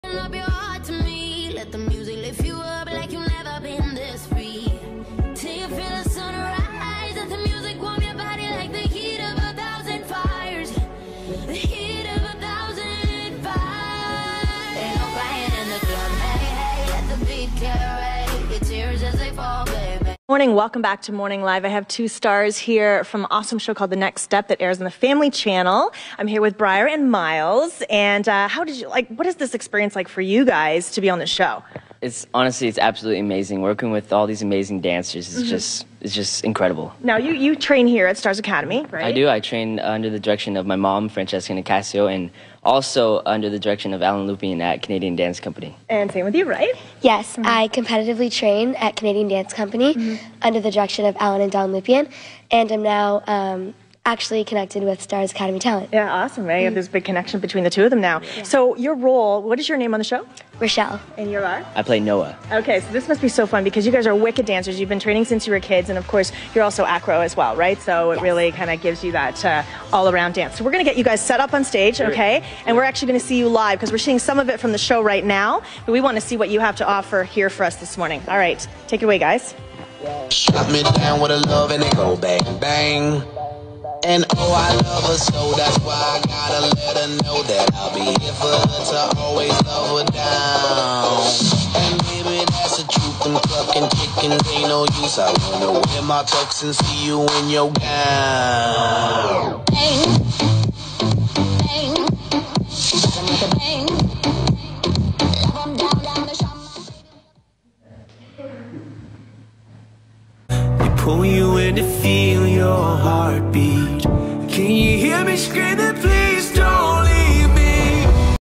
BRIAR AND MYLES | INTERVIEW (Star Academy Talent + Canadian Dance Company)
Star Academy Talent + Canadian Dance Company🤝
I love BRYLES interviews!😹😻
Also another Interview coming out tomorrow of Myles!❤️